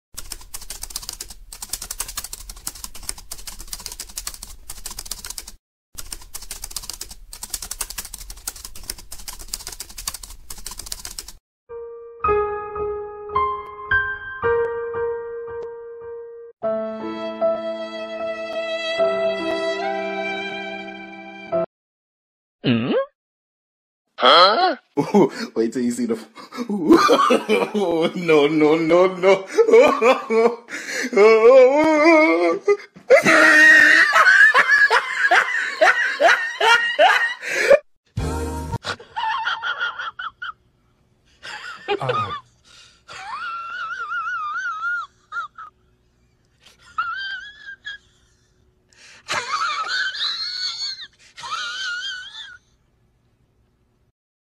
李嘉恩教練開車上路fu系列之268-Shorts Learn Driving Videos來來系列之1-善之本在教，教之本在師。教的如何?也就說出了到底授課如何?
操作轉動方向盤的fu，不只是學一種架勢；練練手感順暢而已。...